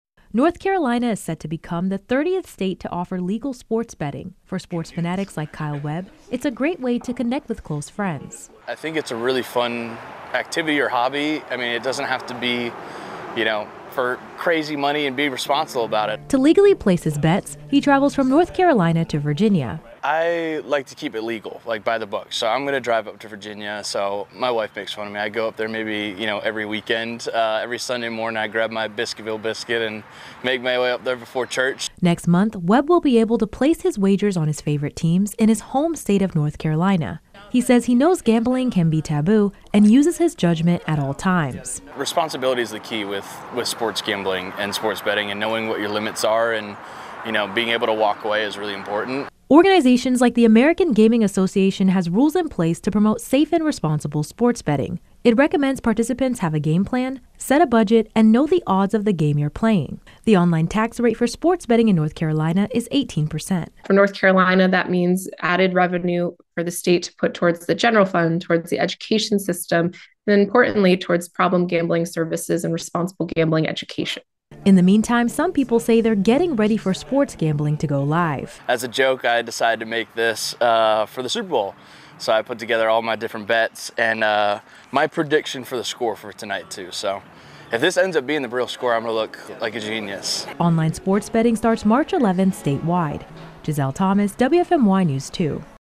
Anticipation builds one month out from sports betting’s launch in North Carolina
Sports gambling goes live in North Carolina on March 11.